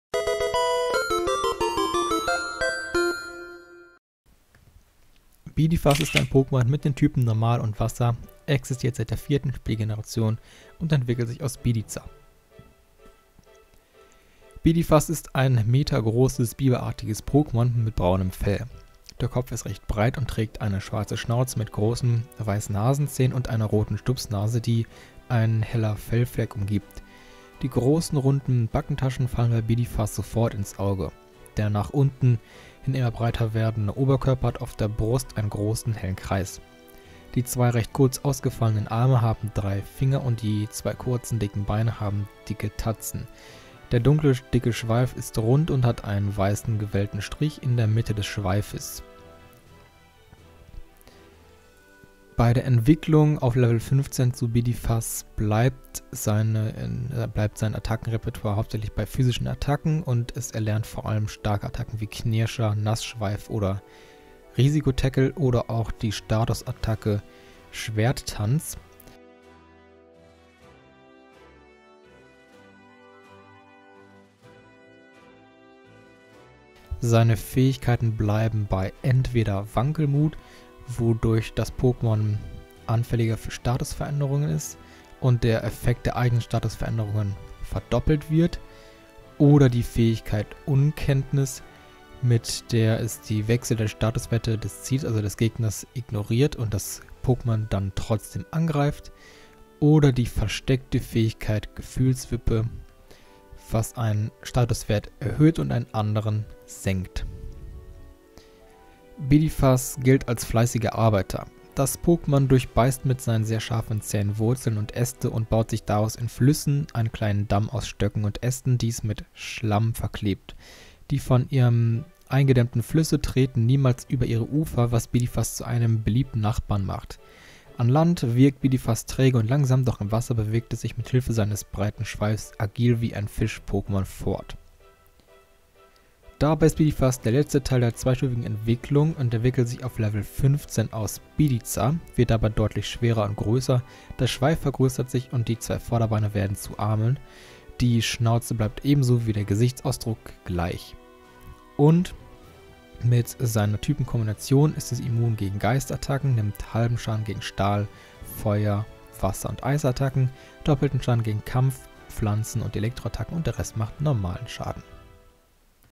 Das Pokémon #400 | Bidifas
▶️Worum gehts in diesem Video:

Ich stelle euch das Pokémon Bidifas vor. Dabei schauen wir auf das Aussehen, Attacken, Verhalten, Lebensraum, Entwicklung und Typ Schwächen. Viel Spaß :D

0:00 Intro
0:14 Aussehen
0:51 Attacken & Fähigkeiten
1:49 Verhalten & Lebensraum
2:15 Entwicklung
2:32 Typ Schwächen